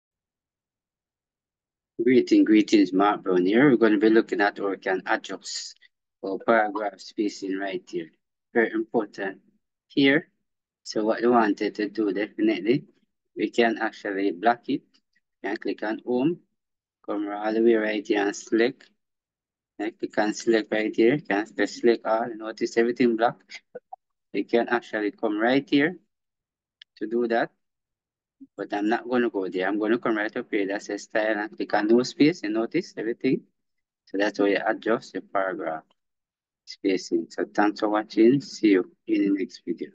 HOW TO ADJUST PARAGRAPH SPACING IN WORD
This tutorial shows how to adjust paragraph spacing in Microsoft Word
Click anywhere in the paragraph you want to change. Go to Layout, and under Spacing, click the up or down arrows to adjust the distance before or after the paragraph. You can also type a number directly.